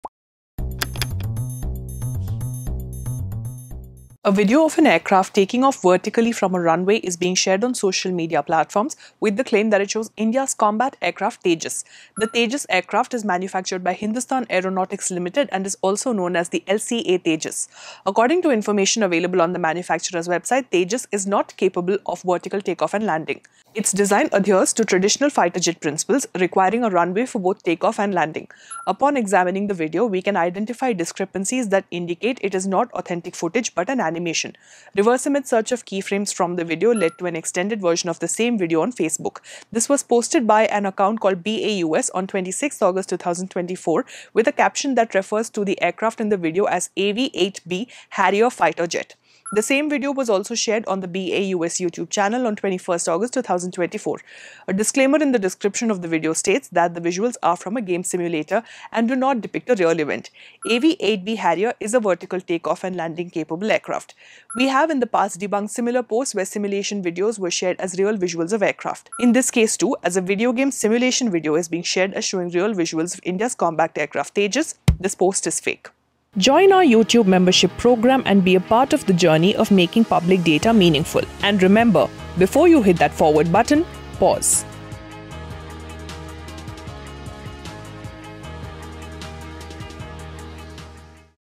FACT CHECK: Viral Video Shows Indian Combat Aircraft Tejas Displaying a Perfect Vertical Take-Off?
A social media post claims that an accompanying video shows Indian combat aircraft Tejas doing a vertical take-off. We check the veracity of the claim in this video.